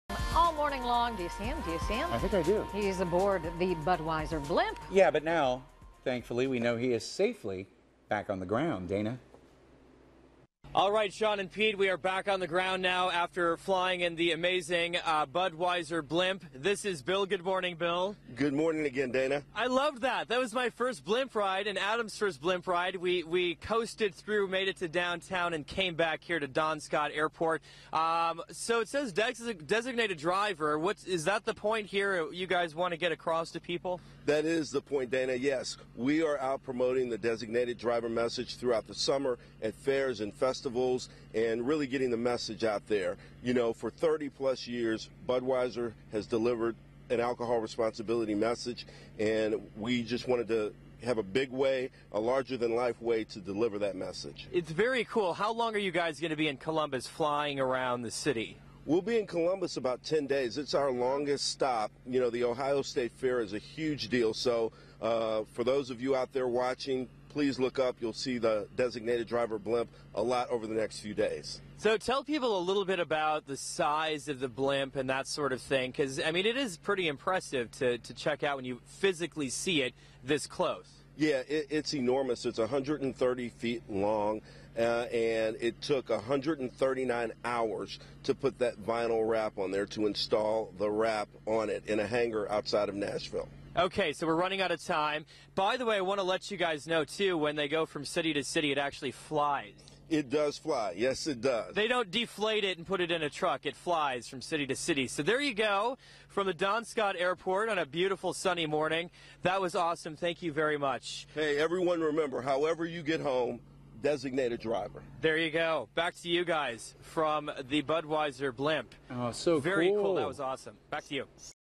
THURSDAY: Dana Up in the Blimp #3
TURTLE AROUND TOWN (Kellie Hanna) Meteorologist Dana Turtle goes up in the Budweiser Designate a Driver blimp this morning.This is his first time in a blimp and it's described as flying/floating in air. The bottom part where all the controls are and where the pilot controls the blimp is called a gondola.  This blimp is here for the Ohio State Fair and is making its longest stop here in Columbus.